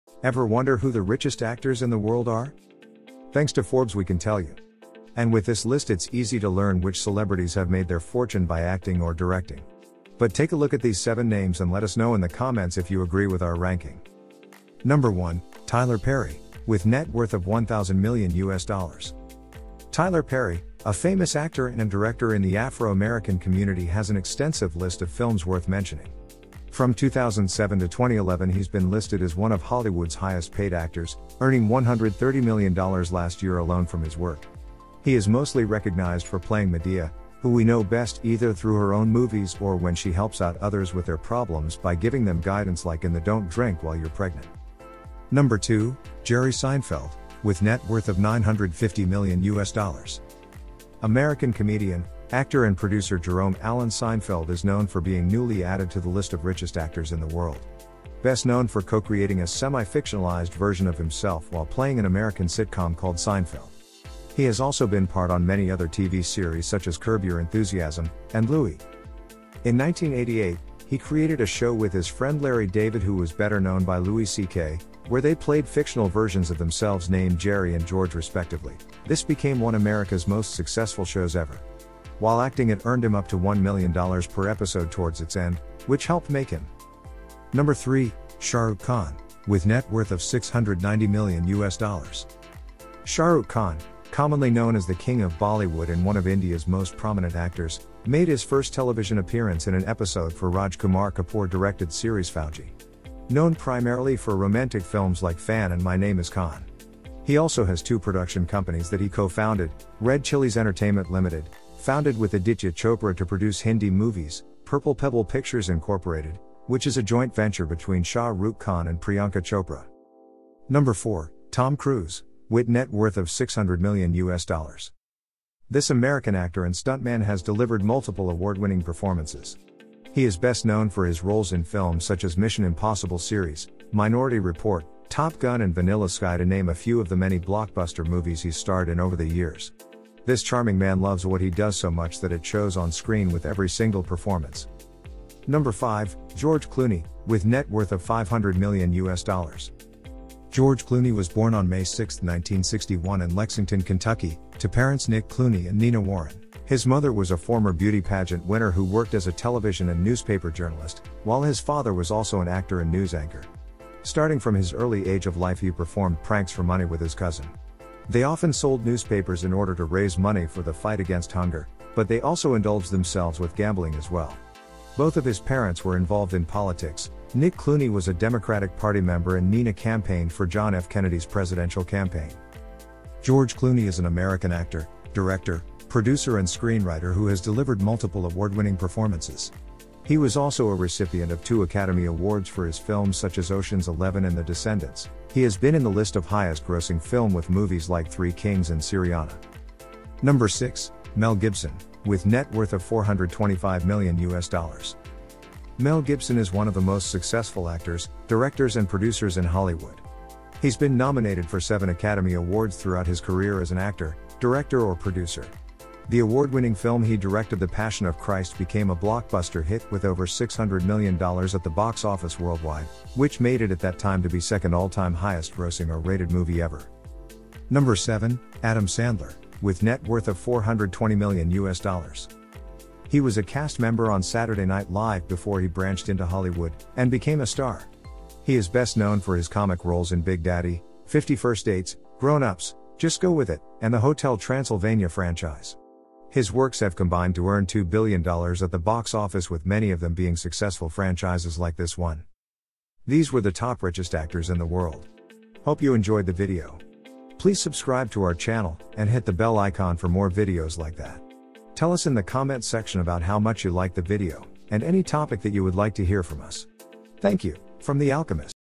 Top Richest /// Wealthiest Actors And Actresses Of Film Industry Along With Net Worth (MUST WATCH)
Actors have been making bank since the days of silent film. But which ones are richest? In this video, I'll share with you a list of the top 10 wealthiest actors in history, and give you some insight into how they earned their fortunes.

Number 1: Tyler Perry, with Net worth of 1000 million USD
Number 2: Jerry Seinfeld, with Net worth of 950 million USD
Number 3: Shahrukh khan, with Net worth of 690 million USD
Number 4: Tom Cruise, with Net worth of 600 million USD
Number 5: George Clooney, with Net worth of 500 million USD
Number 6: ____________________________________________
Number 6: ____________________________________________

Who will be next? Let's watch the video together for that.